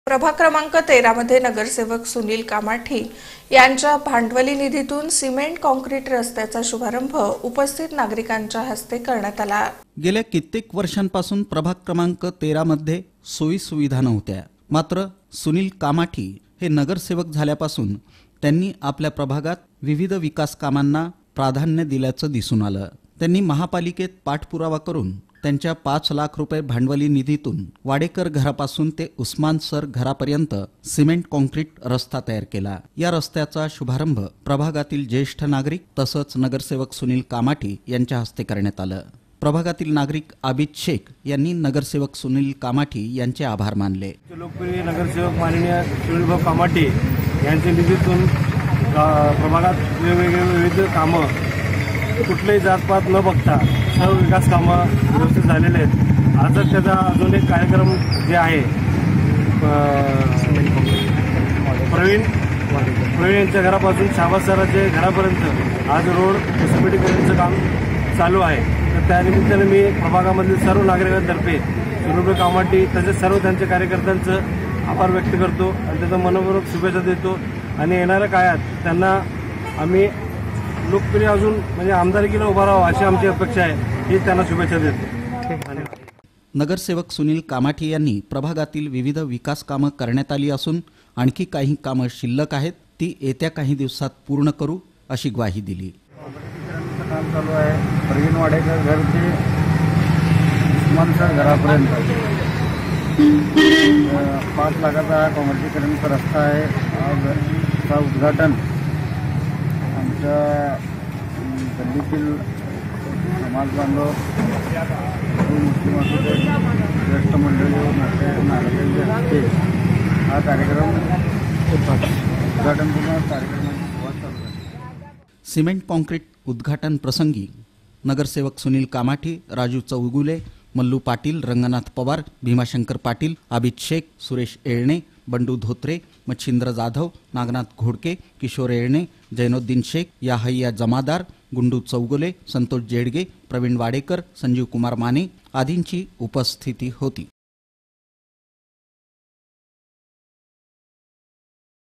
सुनील कामाठी यांचा पुढाकार... प्रभाग १३ सिमेंट रस्त्याचा शुभारंभ... | PR 13 SUNIL KAMATHI
सुनील कामाठी यांचा पुढाकार... प्रभाग १३ सिमेंट रस्त्याचा शुभारंभ...